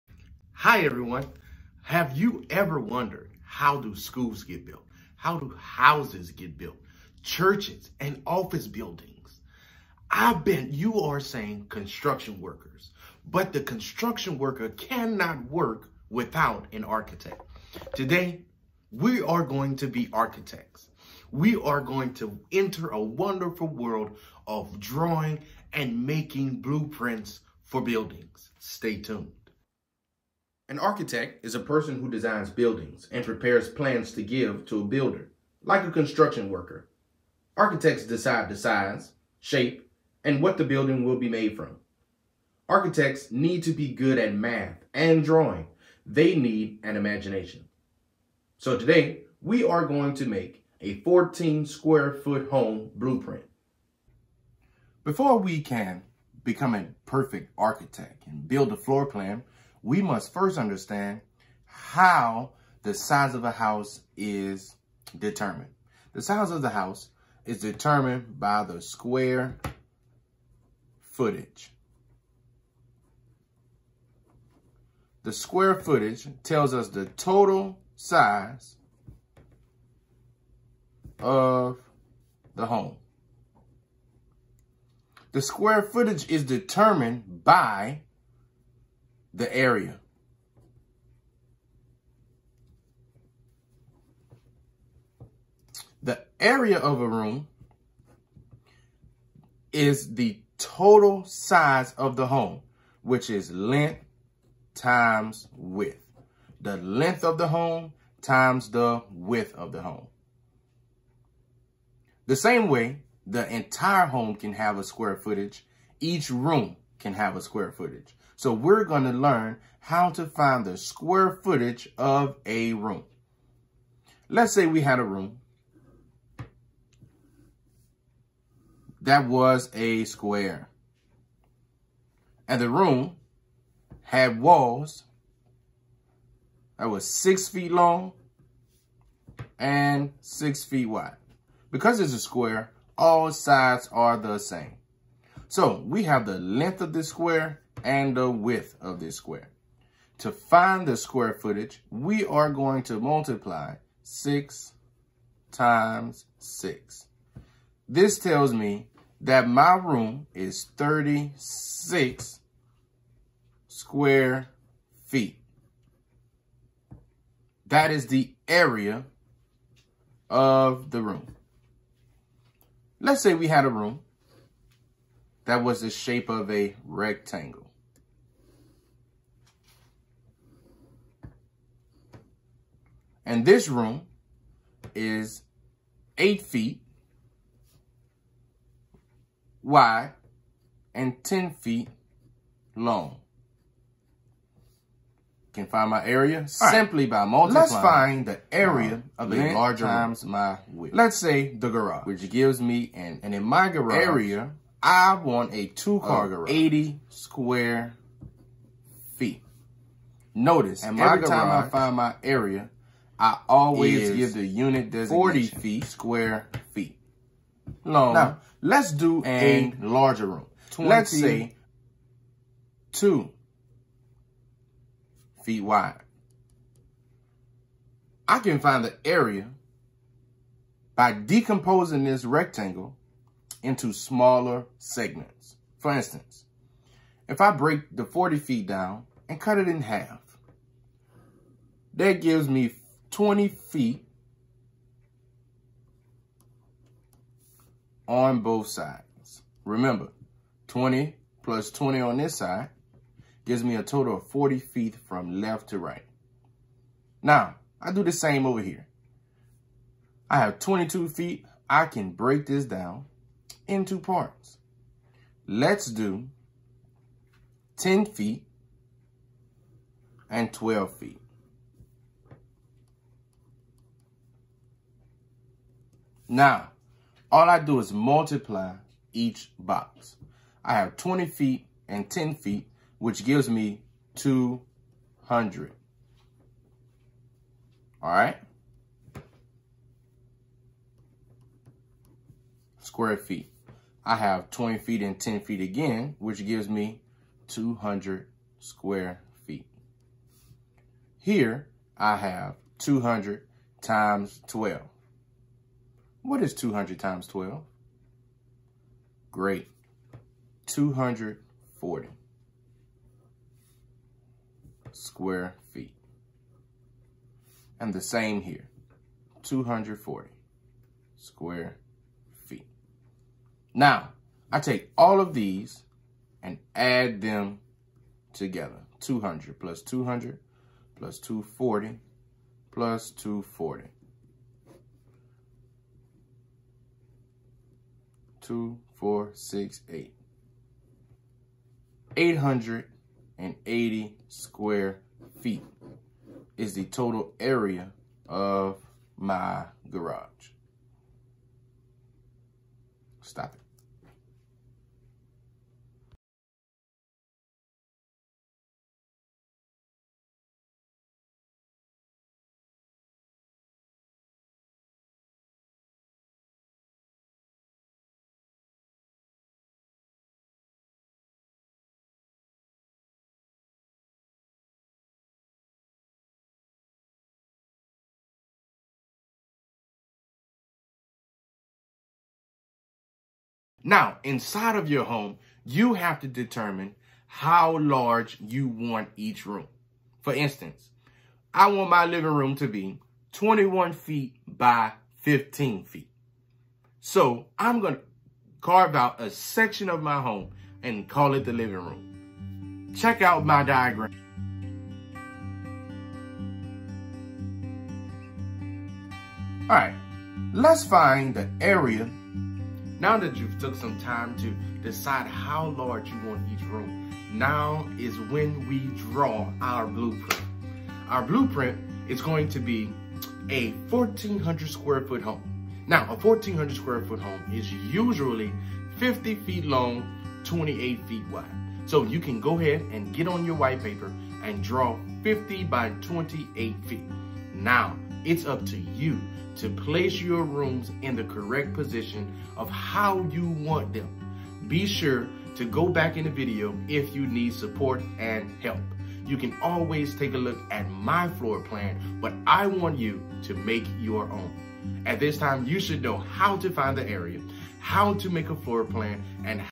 HS Math | Area Floorplan
This lesson with Mr. Jackson gives students real world applications for area and perimeter.